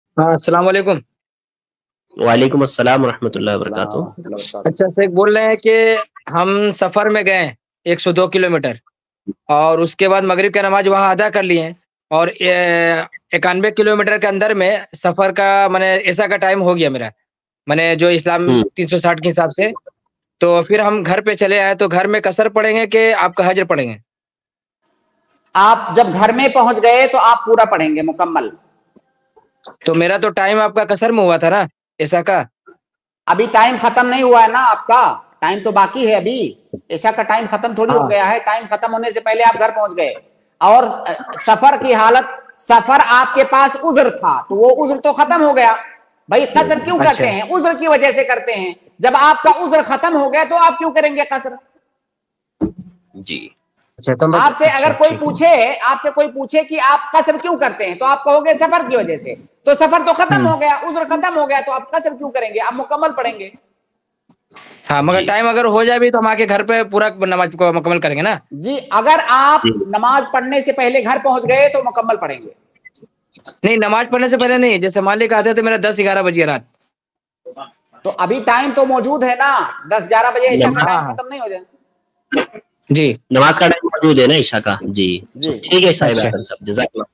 Safar Se Ghar Pahunchne Par Qasr Padhenge Ya Poori Namaz? By Dr. Mohammad Naseem Madani iPlus TV
Safar Se Ghar Pahunchne Par Qasr Padhenge Ya Poori Namaz? By Dr. Mohammad Naseem Madani iPlus TV Deeni Q&A

UPI : donateilm@icici